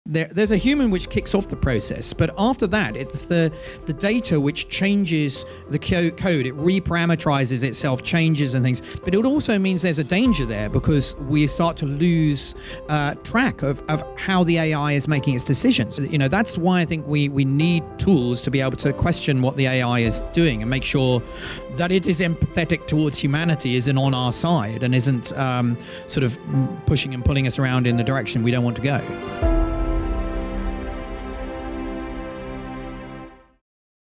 Is AI sentient? Will it write, paint, feel, treat maladies and make us redundant? | Marcus du Sautoy
Is AI sentient? Will it write, paint, feel, treat maladies and make us redundant? Find out in the next fascinating episode of Be Inspired, our series that crystal-gazes into a future of innovation, with zesty and well-known Oxford mathematician Marcus du Sautoy!

Marcus du Sautoy, The Story of Mathematics | Artificial Intelligence: A Powerful Creative in Influence